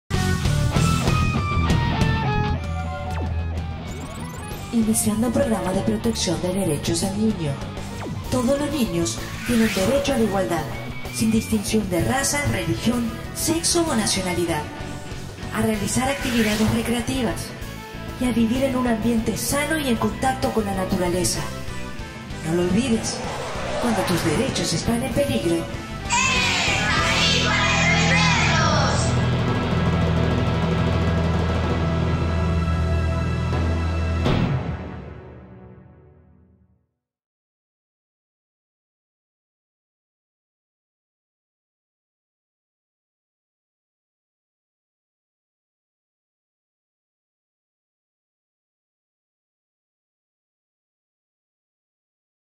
ElDefensor - Programa de Protección de Derecho del Niño 4
➡️Programa de Protección de Derecho del Niño👶🧒👦👧
Todos los niños tienen derechos a:
-La igualdad
-Realizar actividades recreativas
-Vivir en un ambiente sano y en contacto con la naturaleza